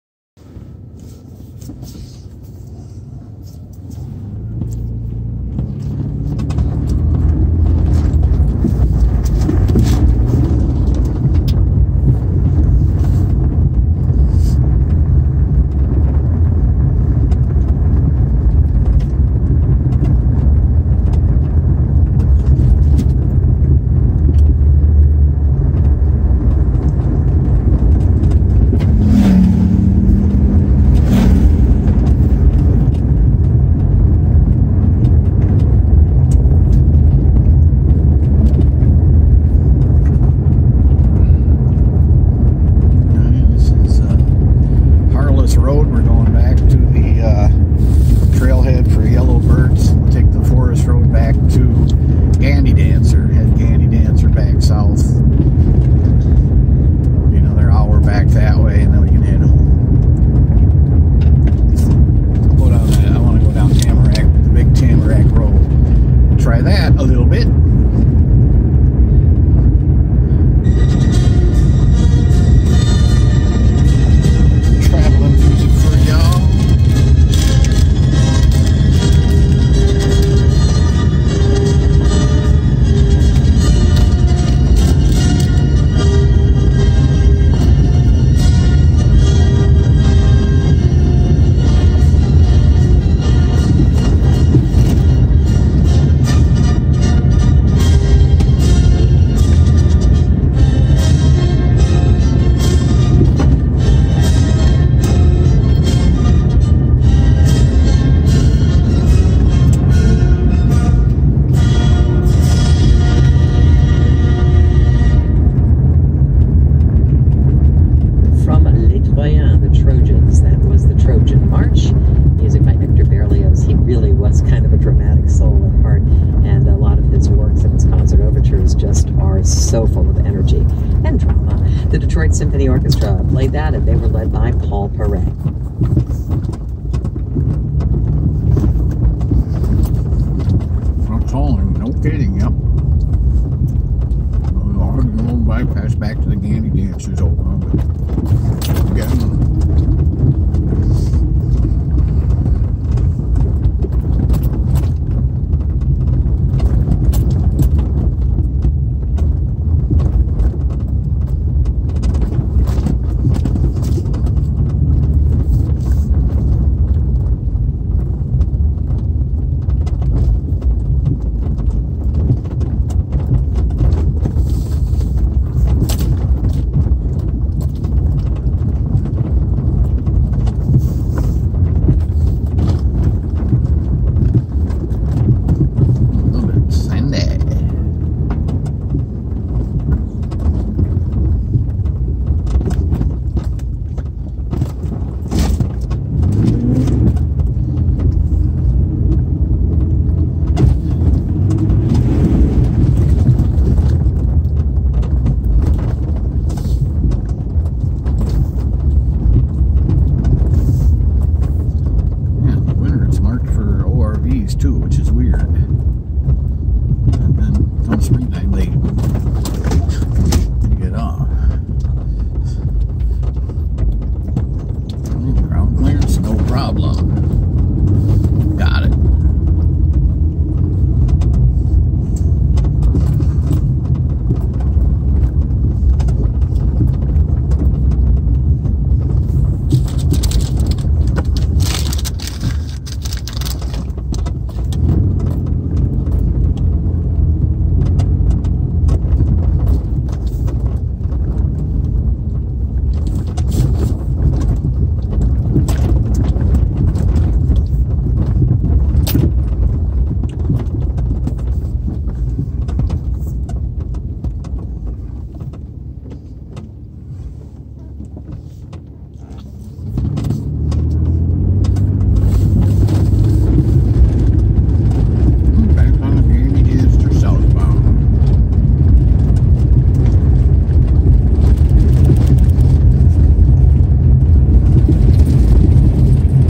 Harlis Rd to Gandy Dancer 5-16-20
Southbound 146 to Harlis Forest Rd to N end of the Gandy Dancer Trail